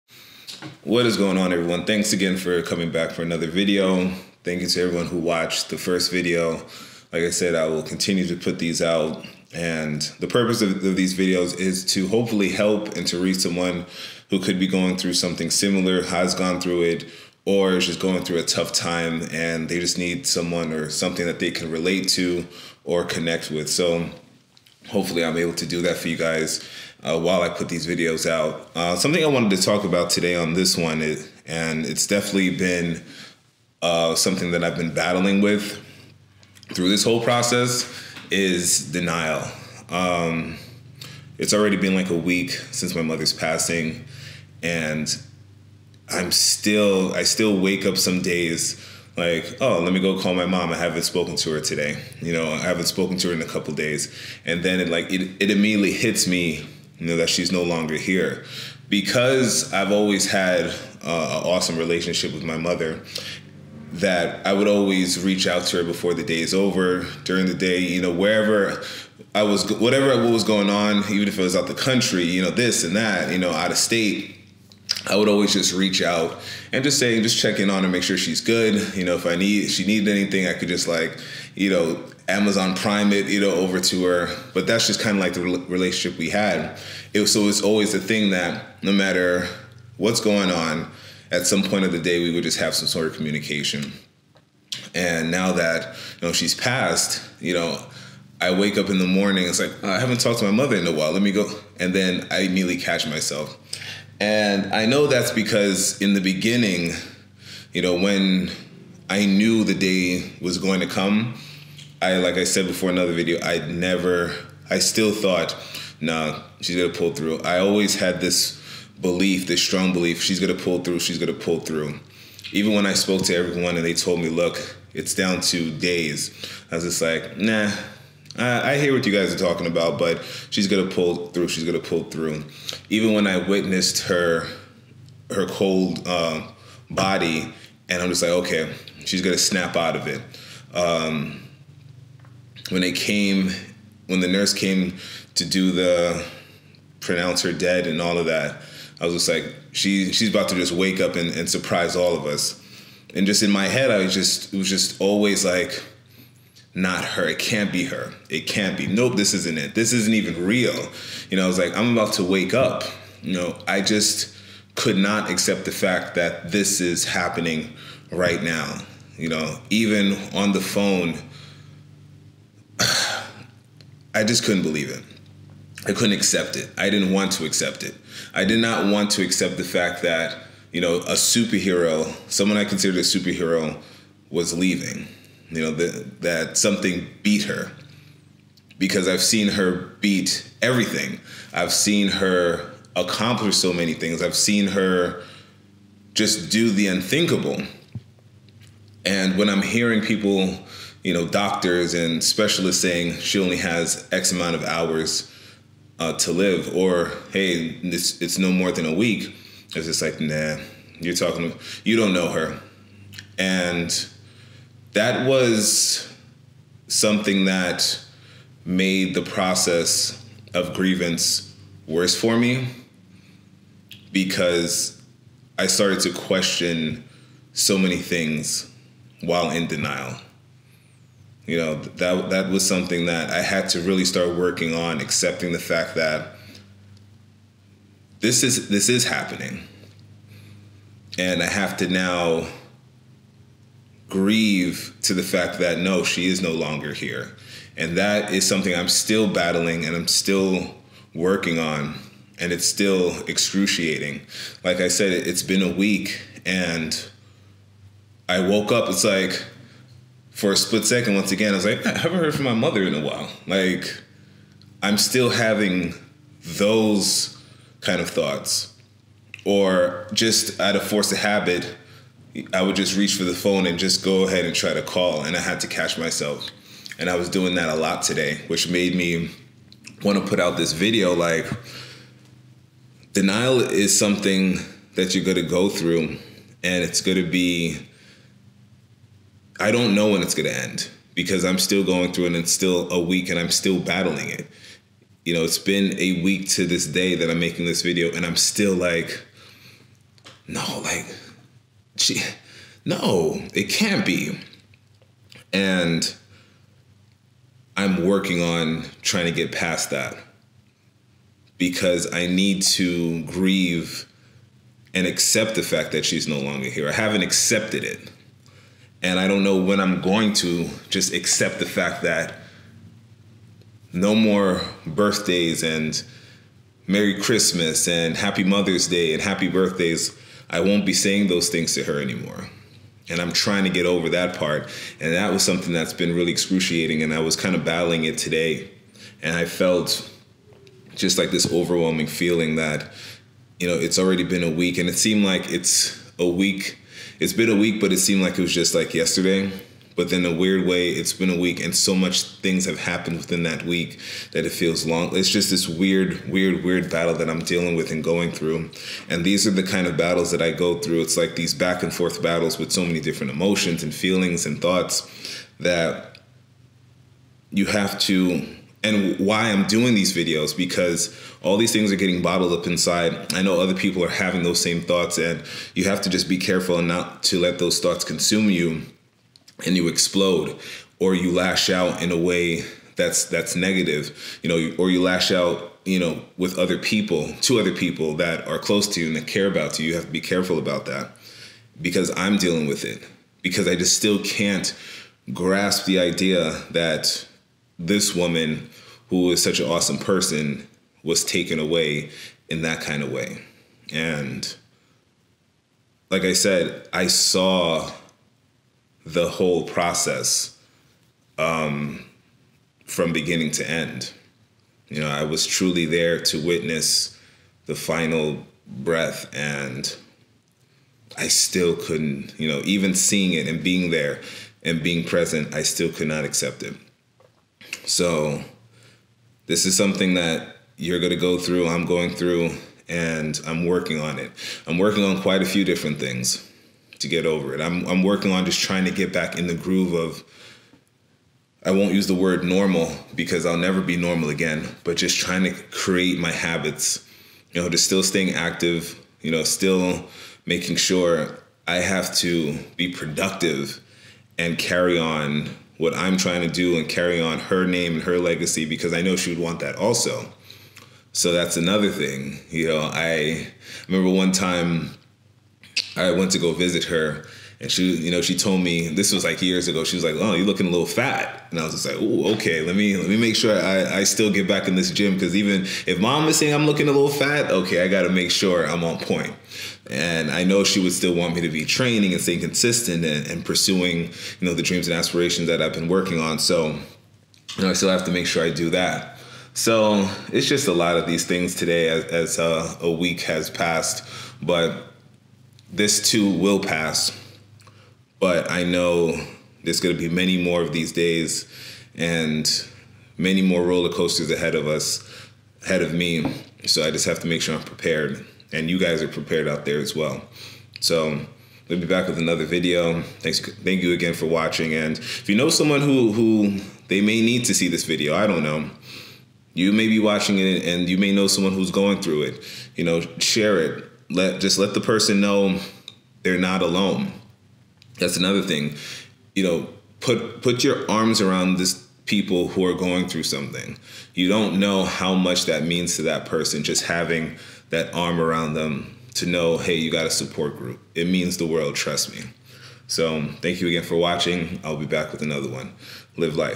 Overcoming denial that my mother is no longer here
You never will truly get over losing someone you love.  You can only try to process it and be at peace the best that you can.  While trying to process it, denial still sets in.  I still reach for my phone to send a text message, I like to see if there is miss call. In my mind, I still feel she's going to reach out or I'm going to wake up from this dream.